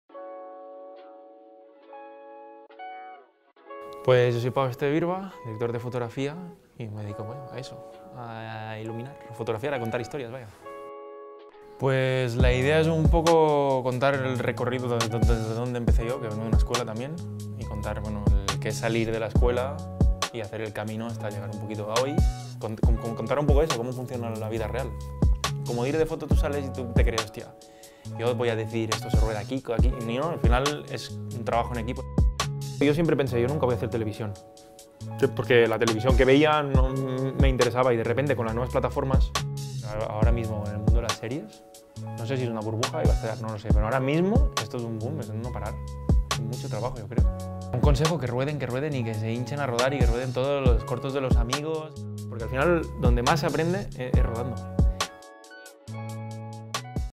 PAU ESTEVE BIRBA - MASTERCLASS DIRECCIÓN DE FOTOGRAFÍA
Pau Esteve Birba, director de fotografía de series actuales como 'La Peste' o 'Arde Madrid', nos ha revelado en primera persona las claves de su profesión y su propia experiencia tanto en cine como en televisión. El encuentro ha sido abierto al público y a los alumnos de nuestro centro.

Ganador del Goya por Mejor Dirección de Fotografía en 2013, por la película 'Caníbal' de Manuel Martín Cuenca, Pau Esteve es a sus 37 años, uno de los mejores cinematógrafos de nuestro país.